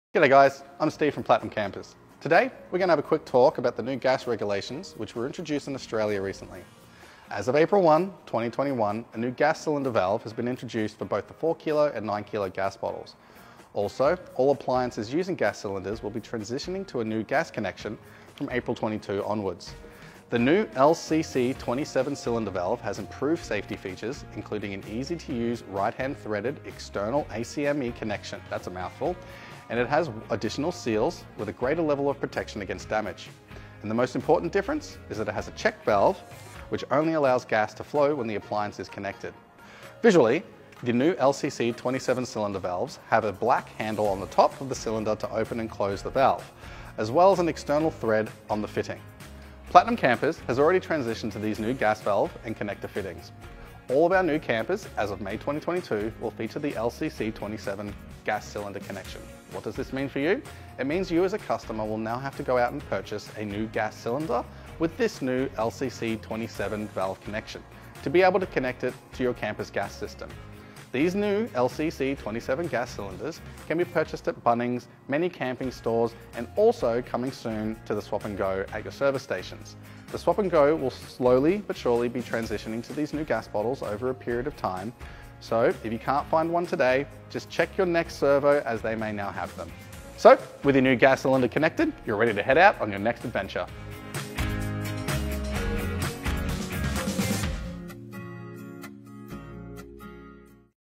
The New LCC27 LPG Gas Cylinder Fittings for Australia
LPG cylinders will be coming out soon with a new fitting called a LCC27. There's a good chance that your current gas bottles use a Type 21 or POL connection.

LCC27 fittings offer improved safety features, including an easy-to-use right-hand threaded external ACME connection, and a back check valve that must be engaged before gas can flow. In addition, there is a thermal cut-out on the LCC27 appliance connection.